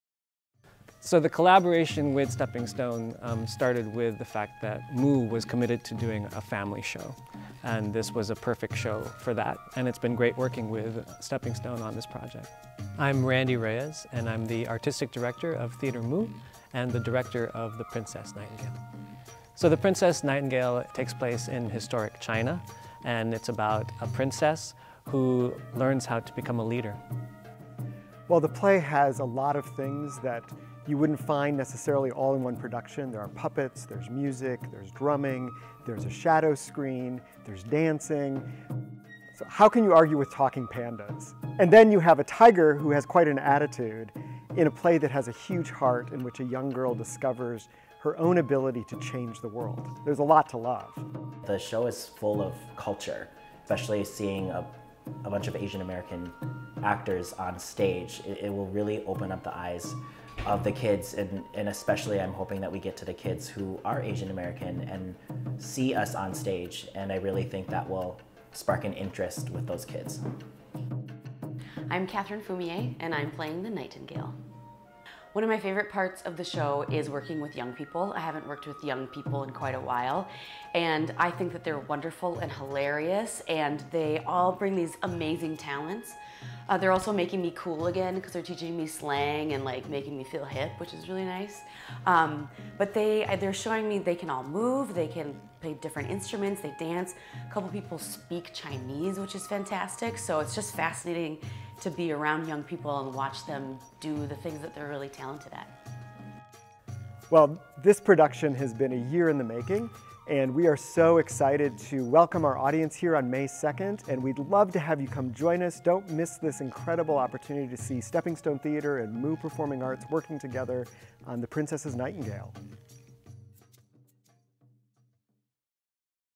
The Princess' Nightingale Trailer
Enjoy a distinctively new adaptation of Hans Christian Andersen's The Nightingale. In classic SteppingStone  style mixed with Theater Mu flair, The Princess' Nightingale  offers an adventure through 18th century China, filled with dazzling music, dance, puppetry, and a heart-felt story of learning to see the world through open eyes.

Performances start May 2nd with official "Opening Night" on May 4th!